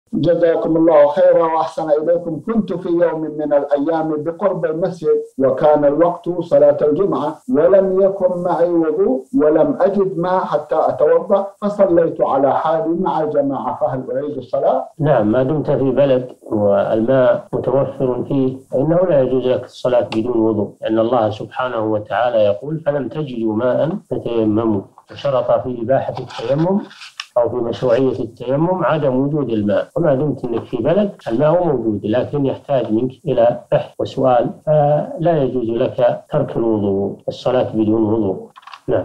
لم أجد ماءا لأتوضأ فصليت على حالي، فما حكم صلاتي؟ الشيخ صالح الفوزان
نص السؤال: كنت في يوم من الأيام بقرب المسجد، وكان الوقت صلاة الجمعة، ولم يكن معي وضوء، ولم أجد ماء حتى أتوضأ، فصليت على حالي مع الجماعة، فهل أعيد الصلاة؟
السيرة الذاتية :
للاشتراك في قناة الشيخ صالح الفوزان

رئيس الفريق
لمشروع كبار العلماء